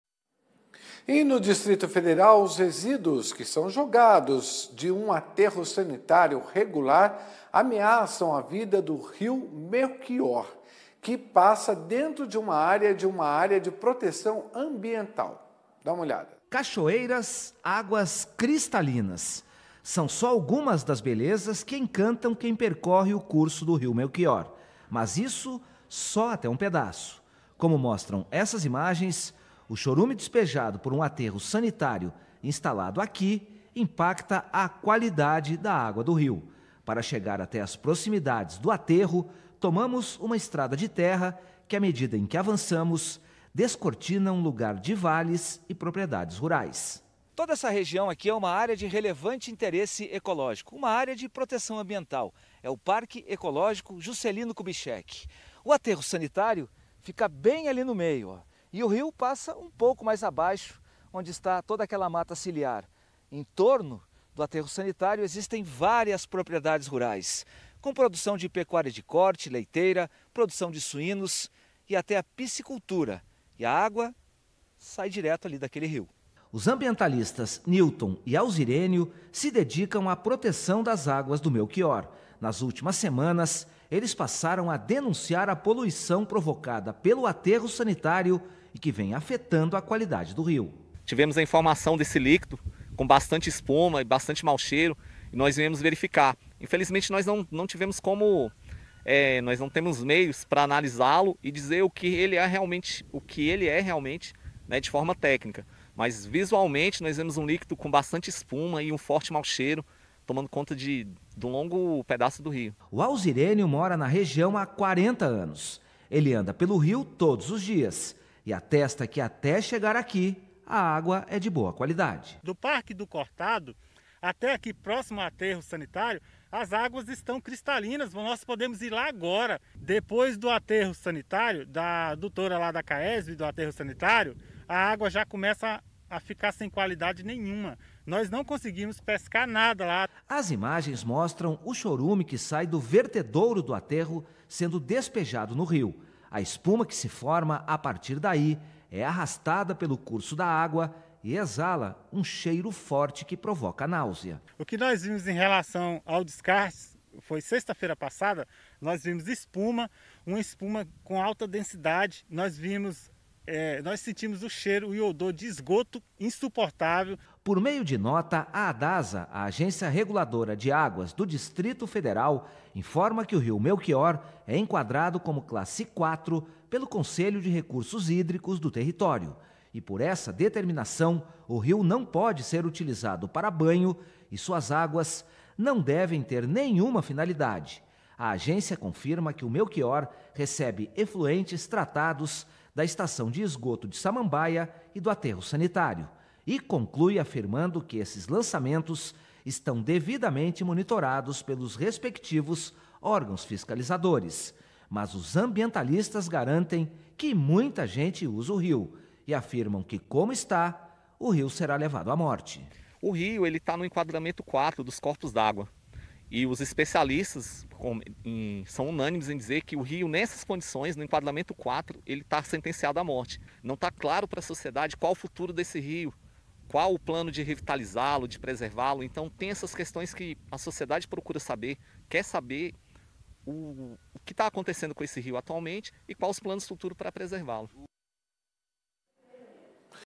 Resíduos de aterro sanitário ameaçam rio no DF l AgroMais
No Distrito Federal, os resíduos que são jogados de um aterro sanitário regular ameaçam a vida do Rio Melchior, que passa dentro de uma área de proteção ambiental.